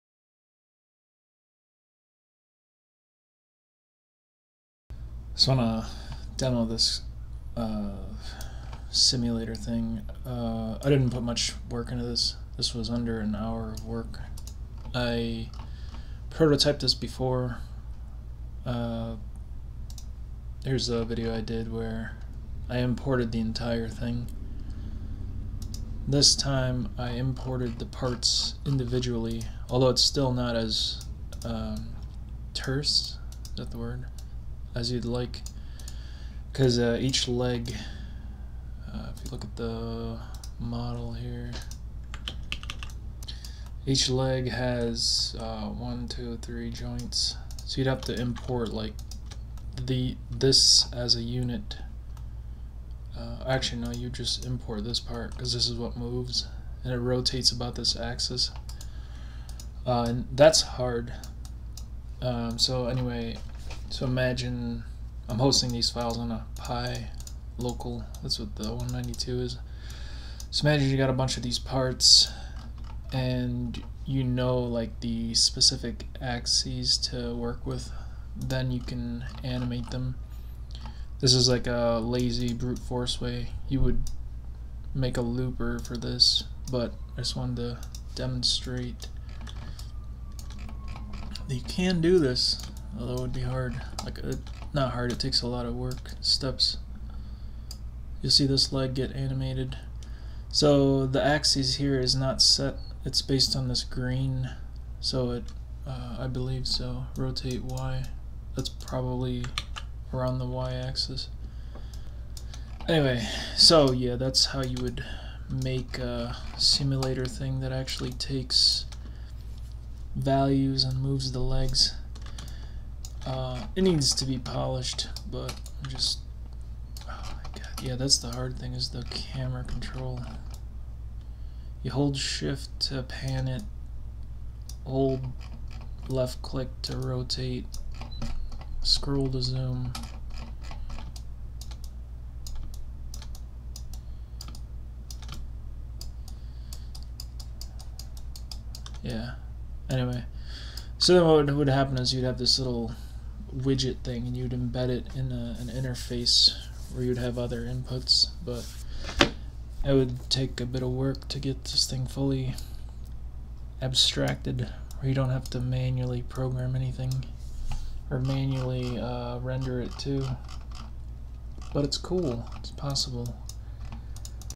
Visual modeling with ThreeJS and SketchUp TLR
Here I did a lazy prototype of animating a 3D model... this one is very simplified... you'd ideally animate the joints (12 parts) to convey a full walk cycle.

But it is pretty cool to do all this with JavaScript